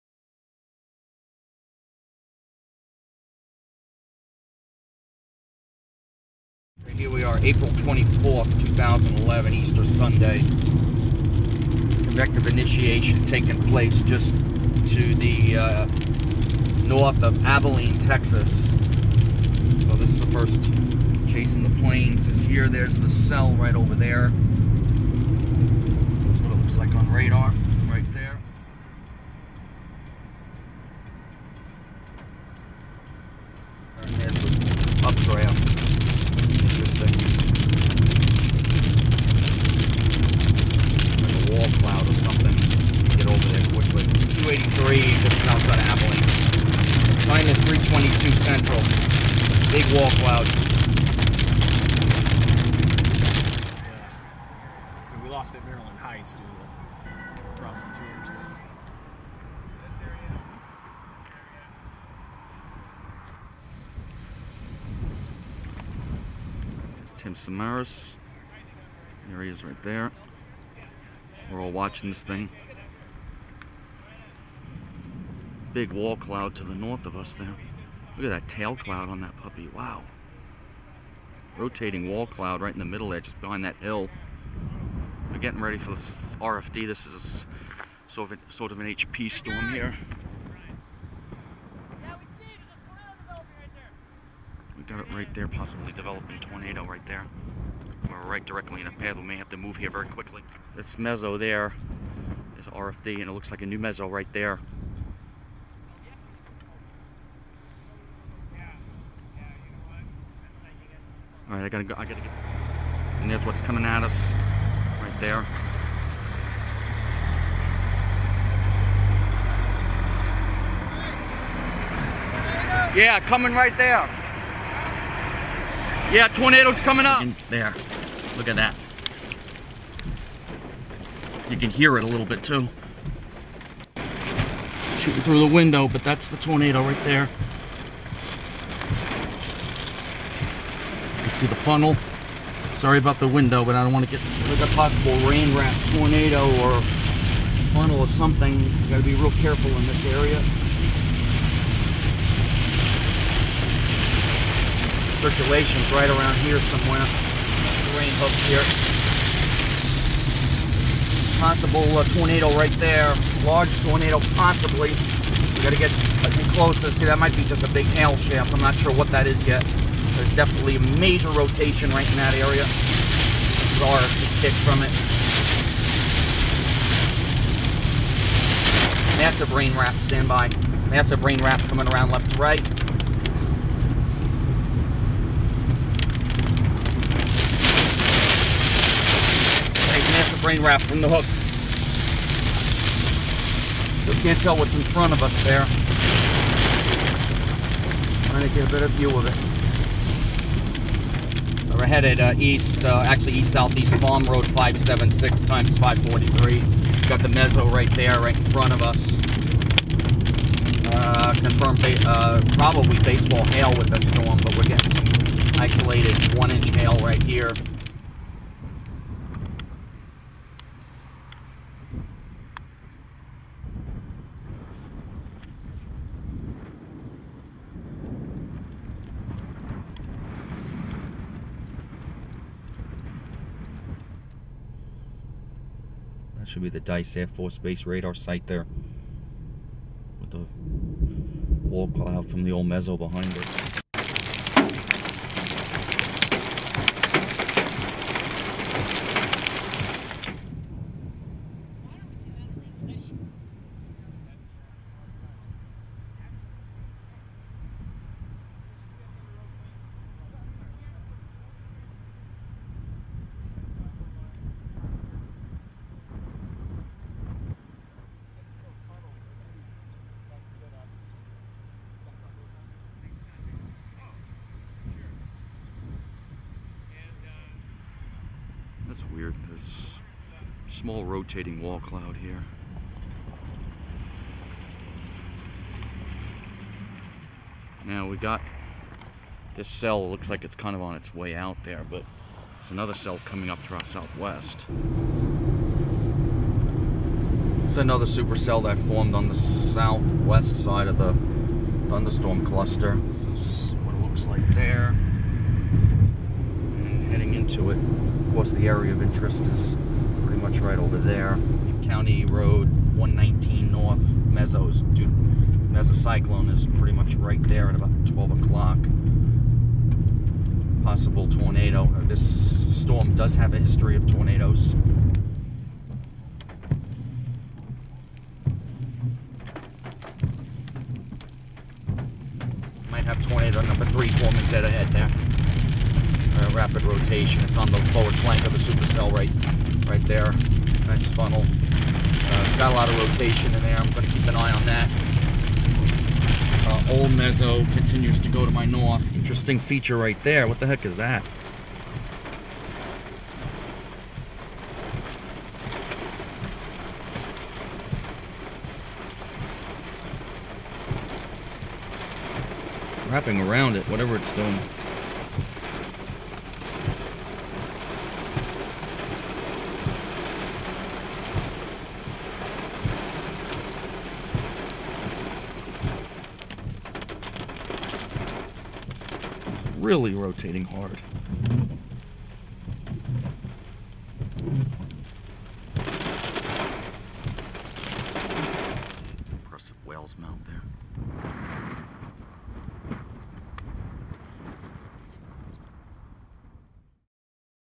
Tornadic Supercells In Texas On April 24, 2011
This is a video of the severe tornadic supercells in the central area of Texas near Baird and Abilene in Callahan County on Easter Sunday (April 24) of 2011. See at least three tornadoes on this quick "spot" chase to tornado alley!